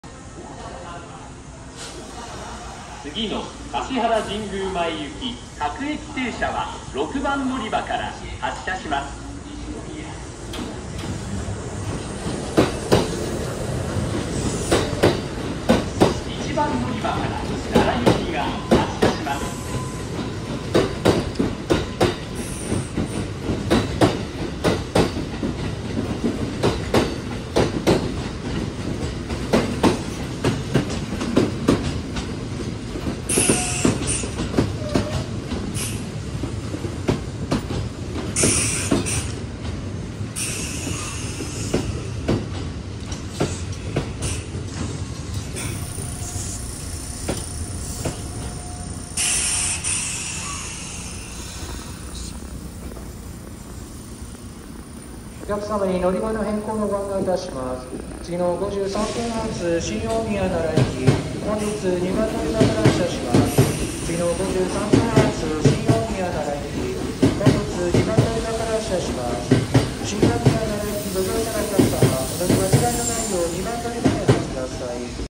近鉄大和西大寺駅を発着する３列車！
日本一ポイントレールが多い近鉄大和西大寺駅
1、2番のりばから発射する西大寺検車区所属5800系6両変形。回送電車として所属先へと引き上げる。田植え機まで大和西大寺行き各駅停車として運転。
2..3番のりばに到着する際たわい事件弱者族9820系8両変形。9時52分発神戸三宮行き快速急行
3.1番のりばから発車する京都市営18系6両変形。9時50分発なら雪急行